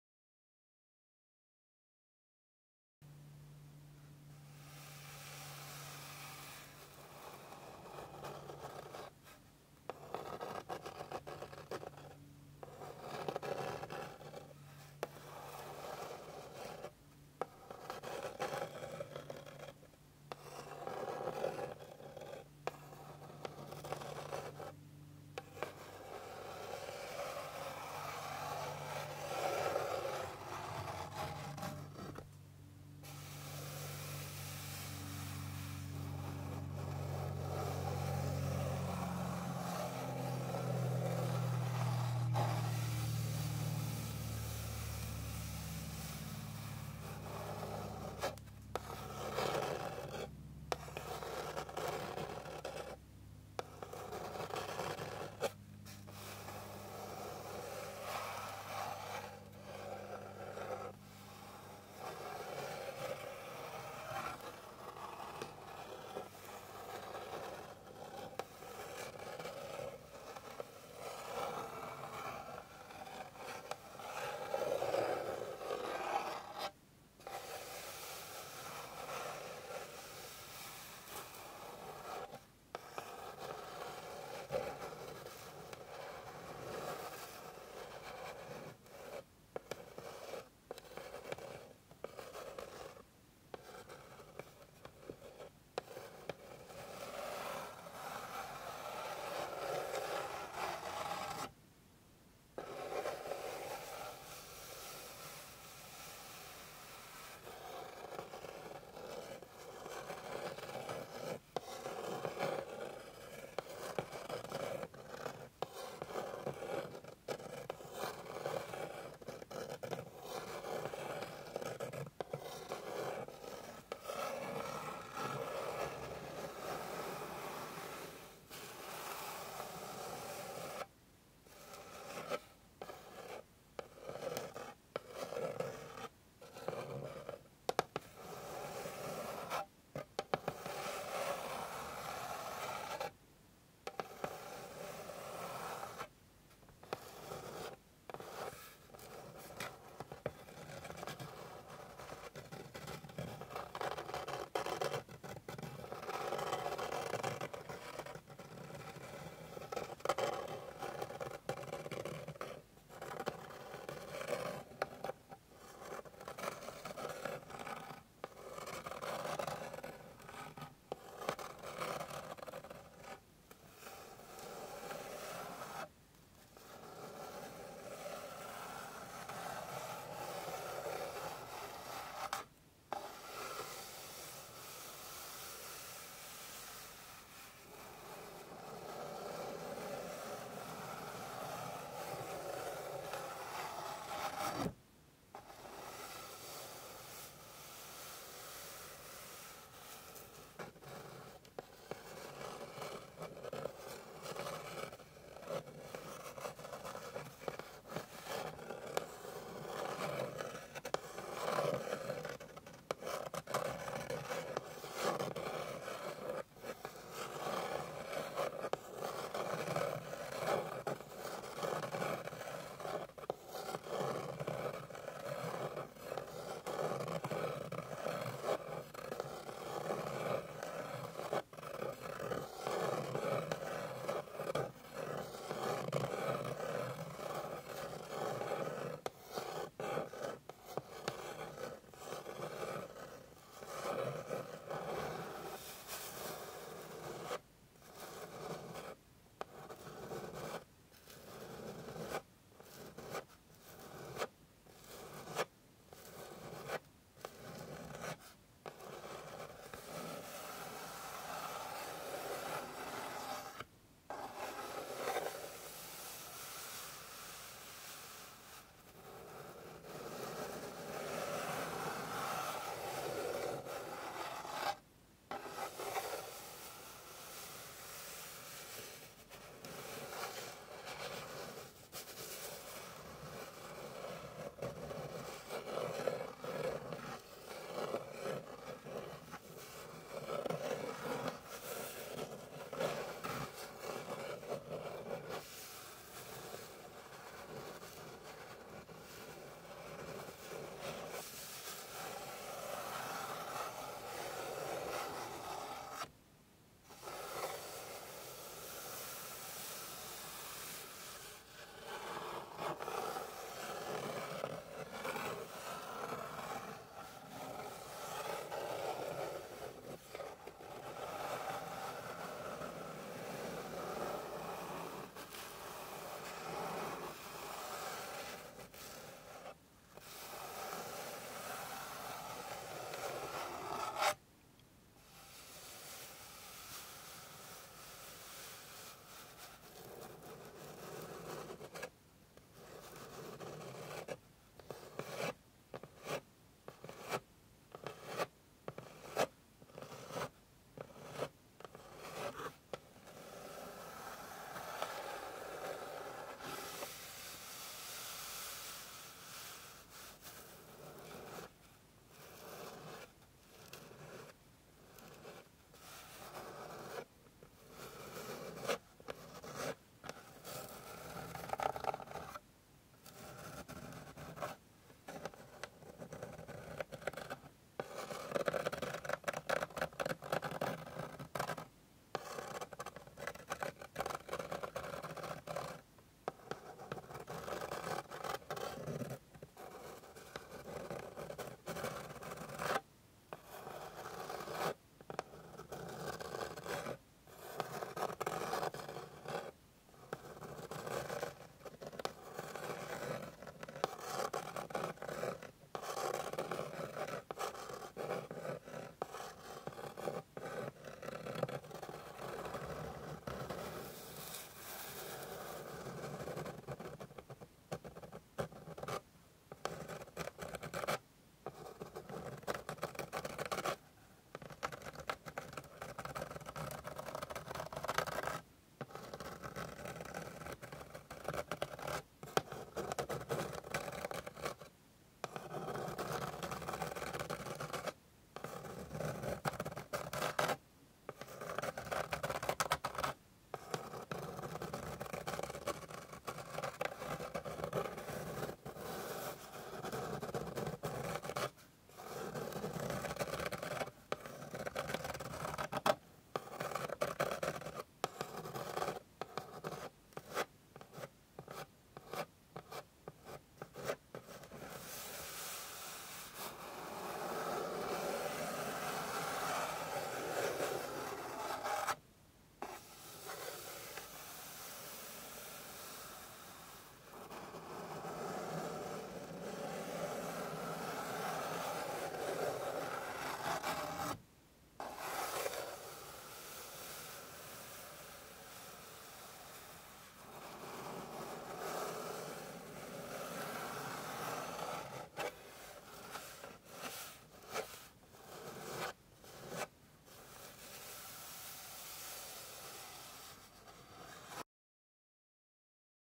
ASMR - Relaxing Woodscratching - No Talking
Hello friends =) I hope you will enjoy my new woodscratching video! =) Have a great night!